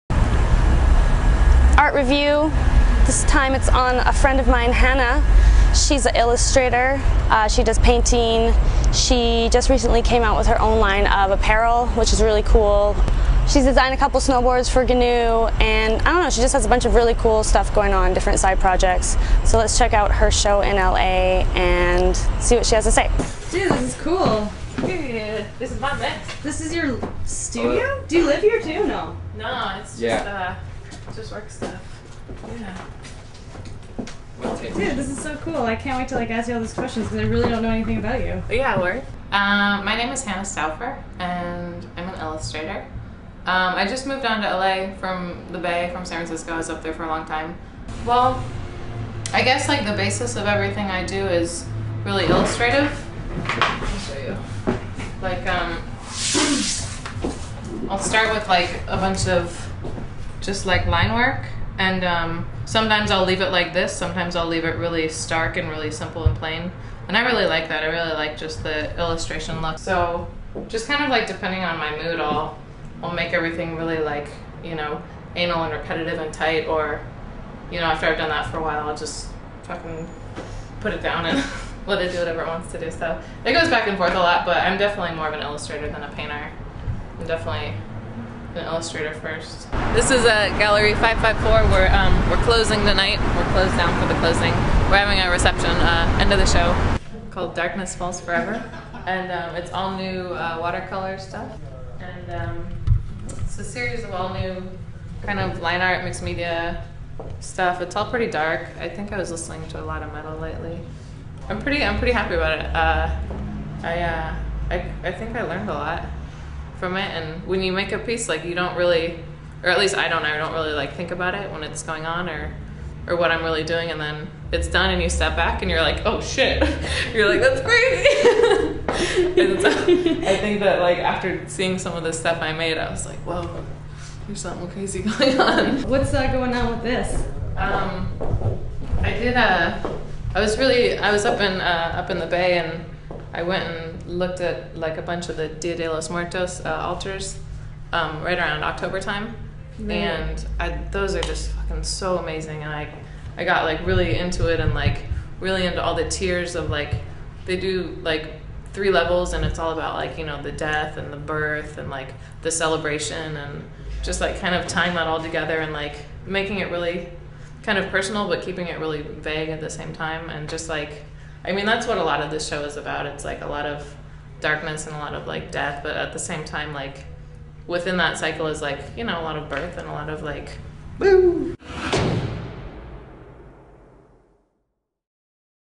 Hannah Stouffer MASHHHHH
Lauren Graham from Jackass World interviews LA based illustrator Hannah Stouffer